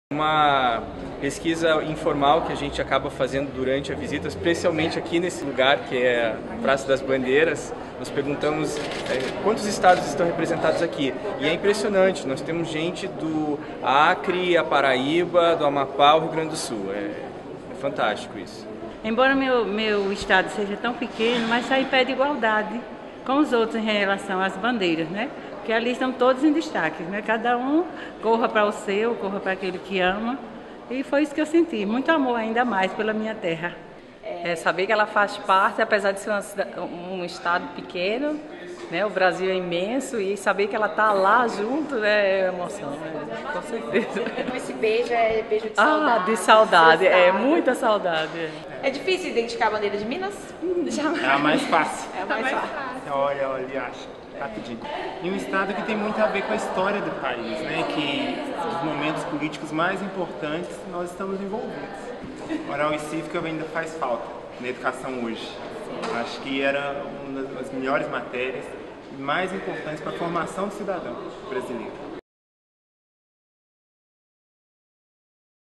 Homenagens a estados na 'praça' das bandeiras marcam visitas ao Senado
Um dos pontos altos do tour oferecido aos visitantes do Congresso Nacional é a parada em frente ao conjunto das bandeiras dos 26 estados e do Distrito Federal, no Salão Azul do Senado. Reunidos em frente ao Plenário, os 27 pavilhões recebem homenagens, às vezes bastante emocionadas, de cidadãos de várias partes do país. A reunião das bandeiras simboliza o papel do Senado, a Casa da Federação, criado para representar os estados, em contraponto com a Câmara dos Deputados, que representa a população.